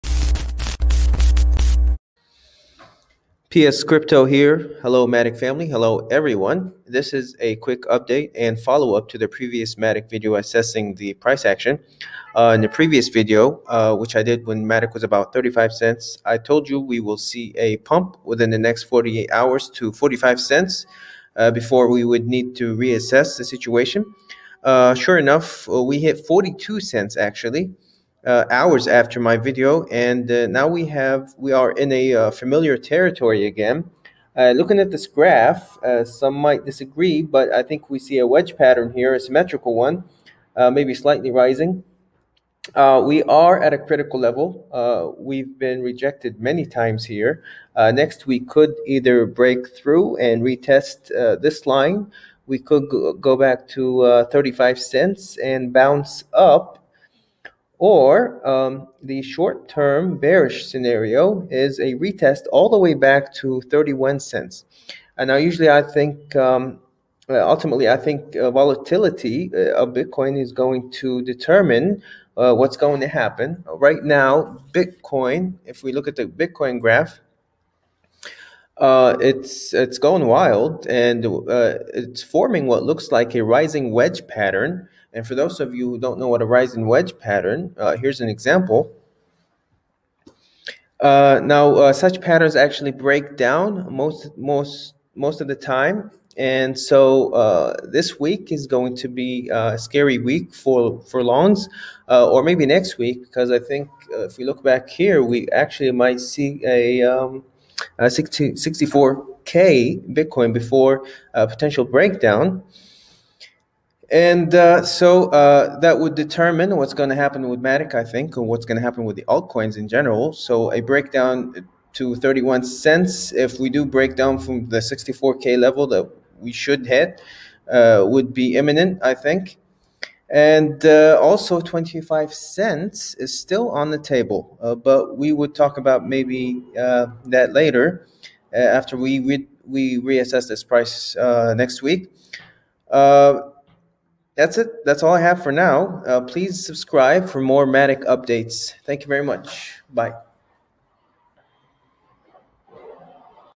Polygon Matic Network price update - multiple scenarios + Bitcoin Price Analysis
matic network
bitcoin price